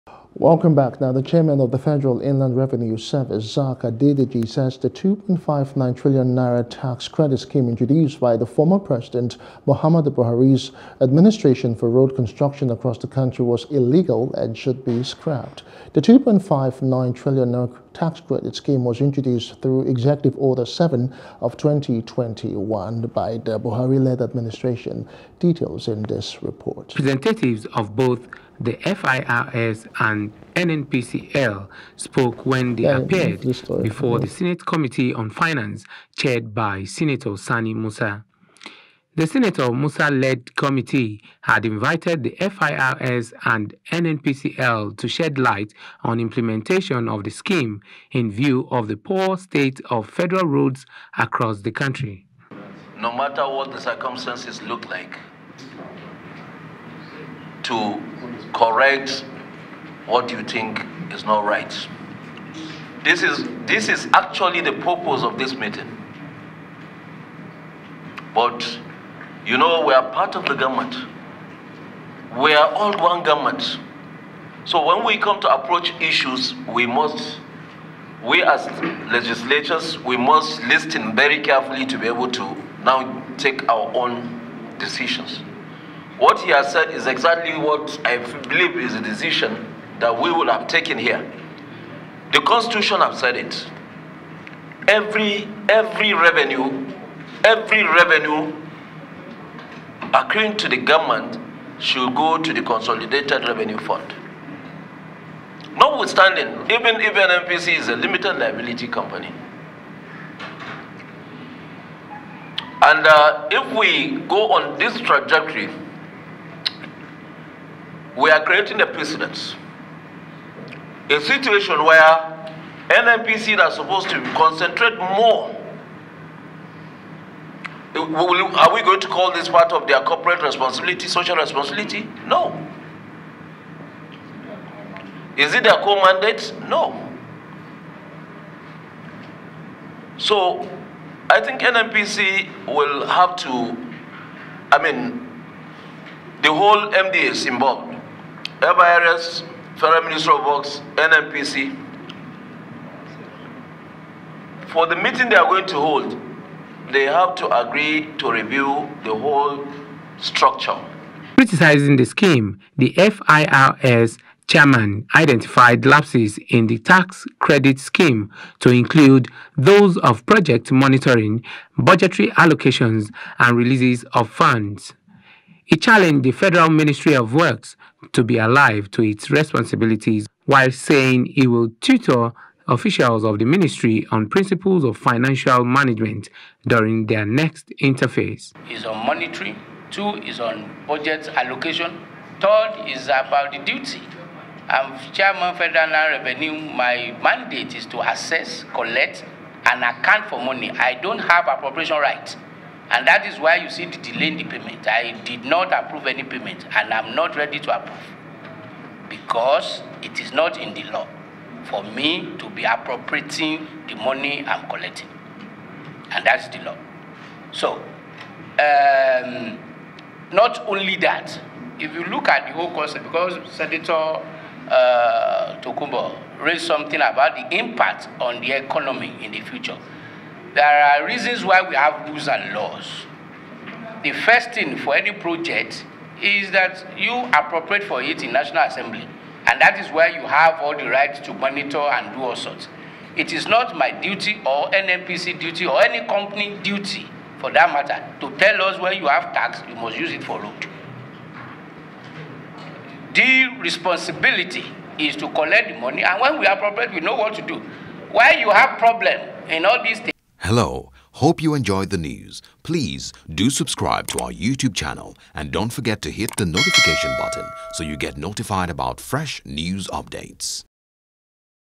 Road Construction Senate, Firs Fault N2 59trillion Tax Credit Scheme
The Chairman of Federal Inland Revenue Service ( FIRS), Zacch Adedeji, says the N2.59 trillion Tax Credit Scheme introduced by former President Muhammadu Buhari’s administration for road construction across the country was illegal and should be scrapped.